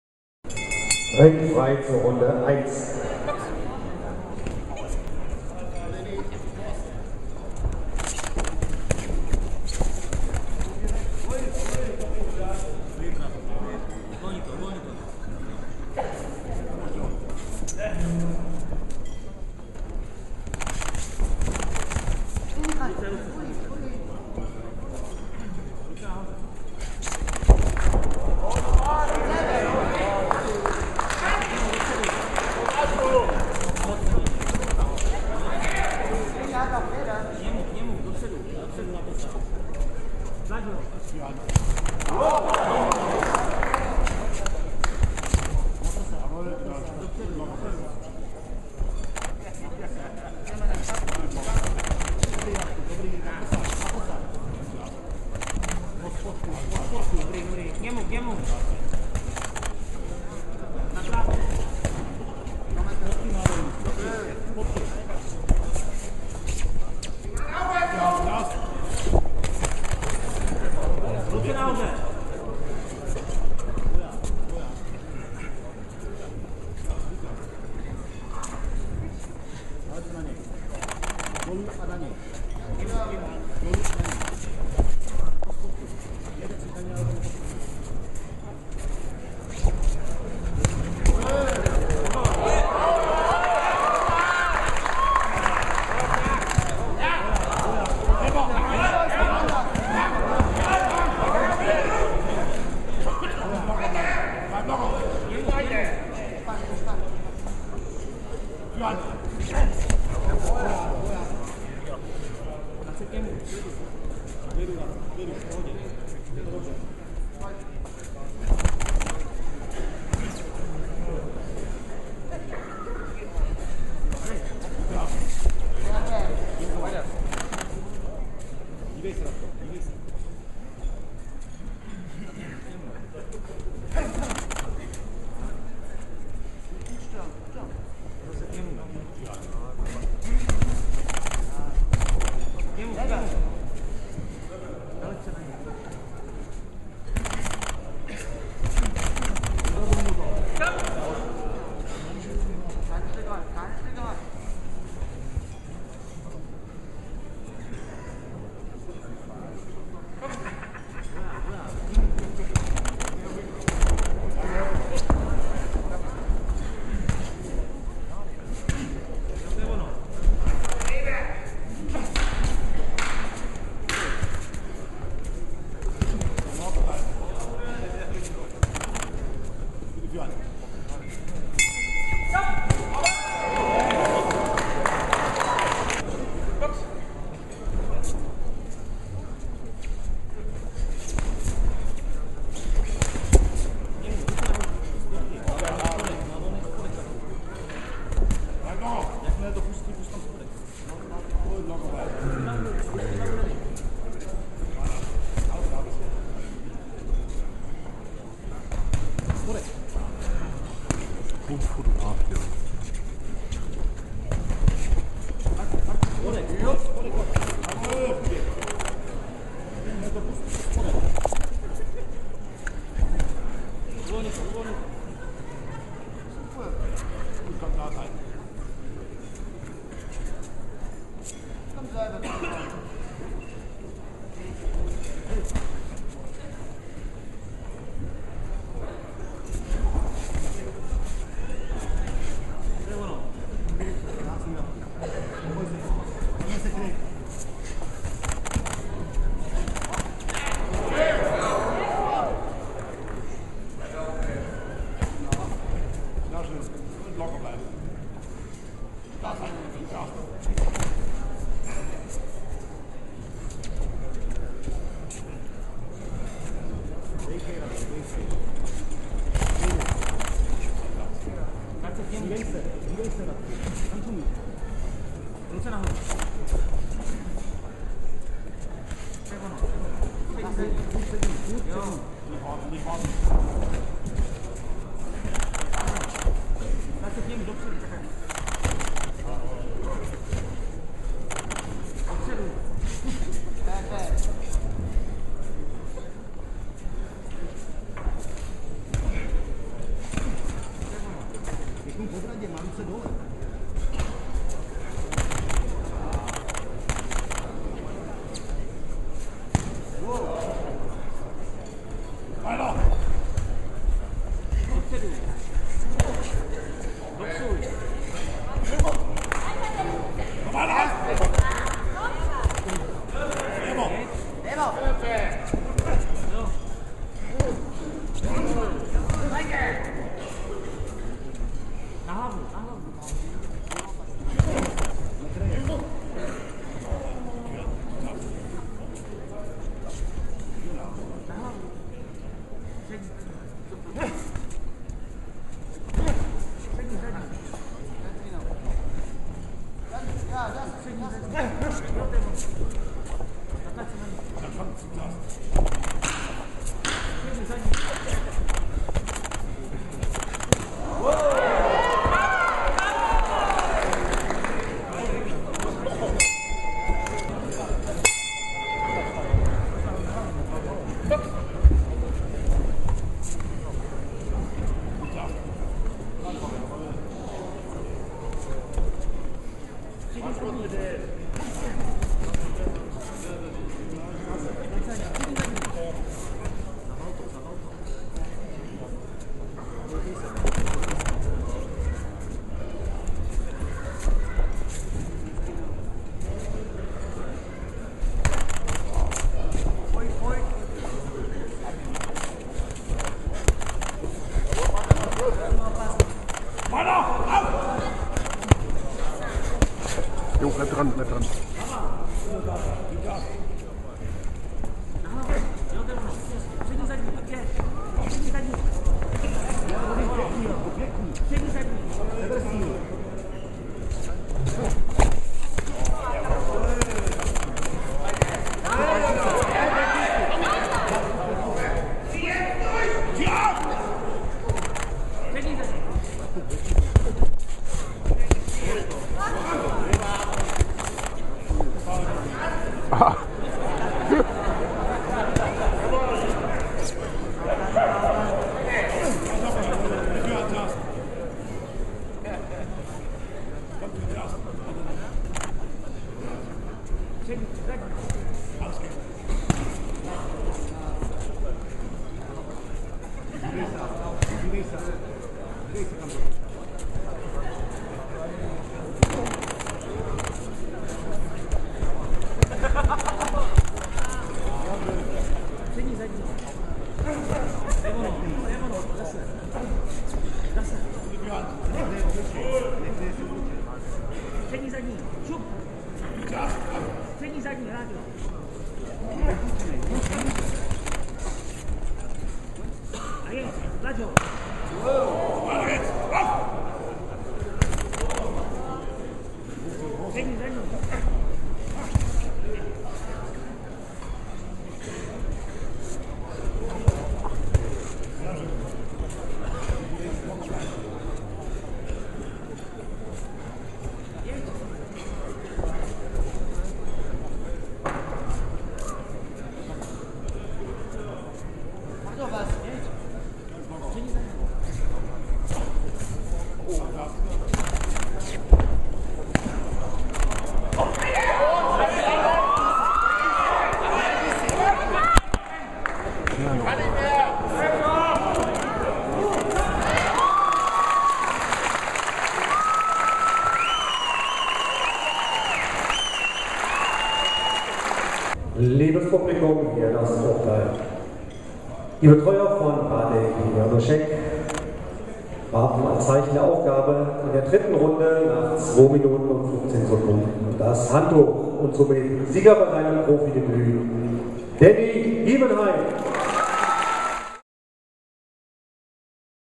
Dany Giebenhain vs Radek Janousek
Dany Giebenhain won his debut as a pro boxer against Radek Janousek
Schwerin, 17th February 2013

Dany Giebenhain gewinnt seinen ersten Kampf als Profiboxer gegen Radek Janousek
Schwerin, 17. Februar 2013